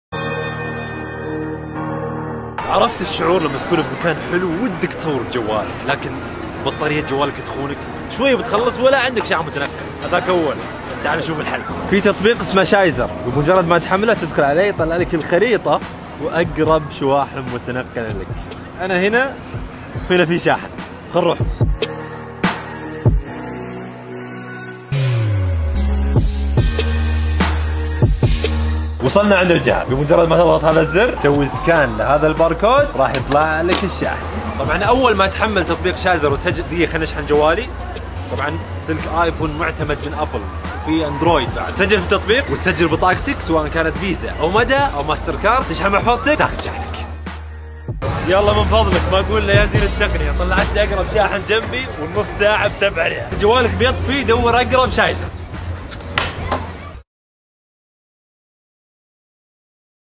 بدر السعد يشرح في ونتر وندرلاند ما هو شايزر وكيف تلقى أقرب جهاز لك وتستأجر بطارية.. ولا يطفى جوالك
بدر السعد يشرح في ونتر وندرلاند ما هو شايزر وكيف تلقى أقرب جهاز لك وتستأجر بطارية.. ولا يطفى جوالك وخلك شابك